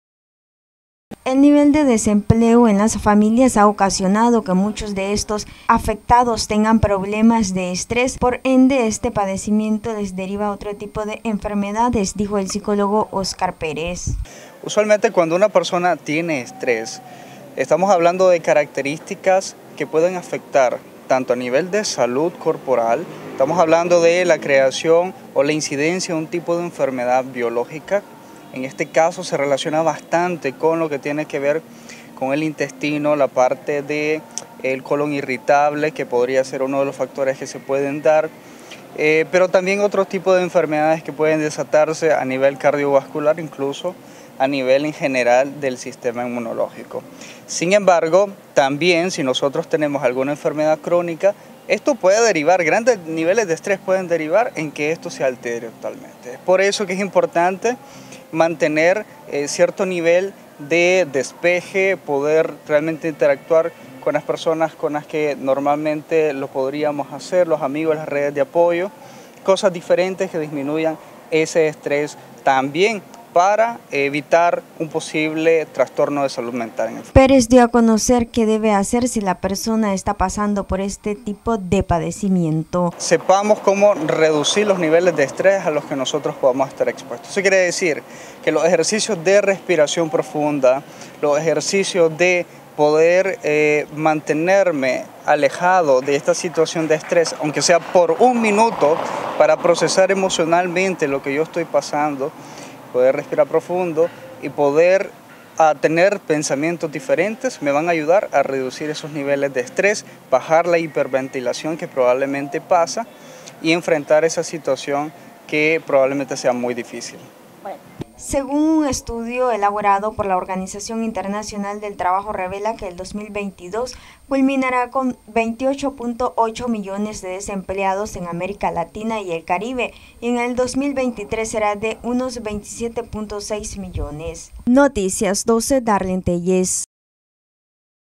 Personas continúan teniendo problemas de estrés por diversos factores
Noticias12Nicaragua El psicólogo Òscar Pérez manifestó que muchas personas están padeciendo de problemas de estrés en la que muchos casos se debe a los problemas del desempleo.